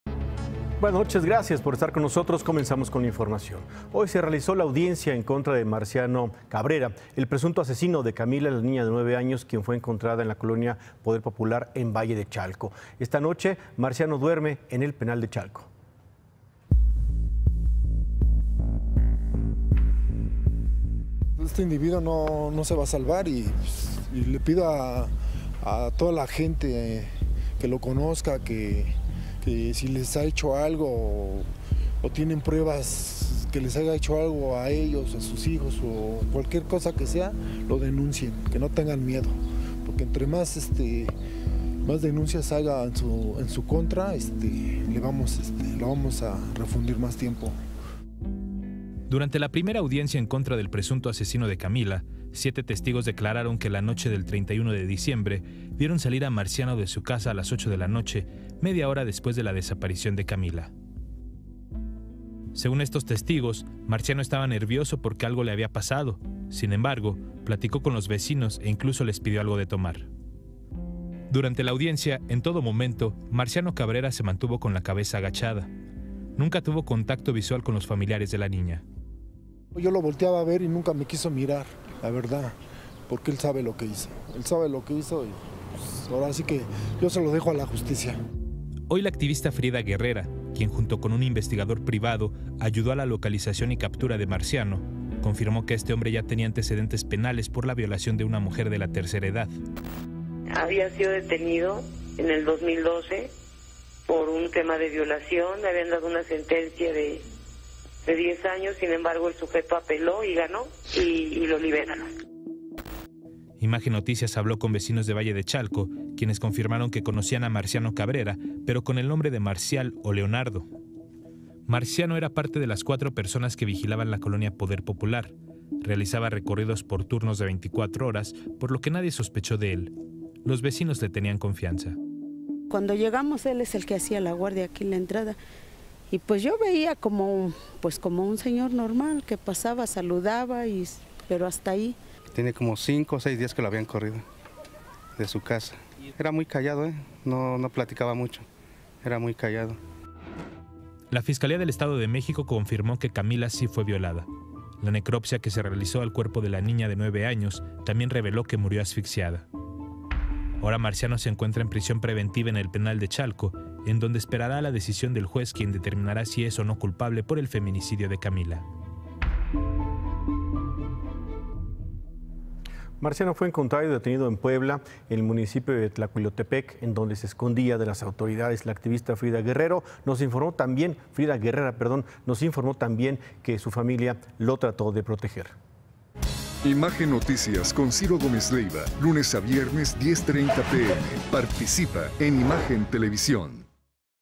Imputan feminicidio a Marciano Cabrera por la muerte de la niña Camila en Valle de Chalco
Hoy se llevó a cabo la primera audiencia de vinculación a proceso del asesino de la niña Camila en Valle de Chalco.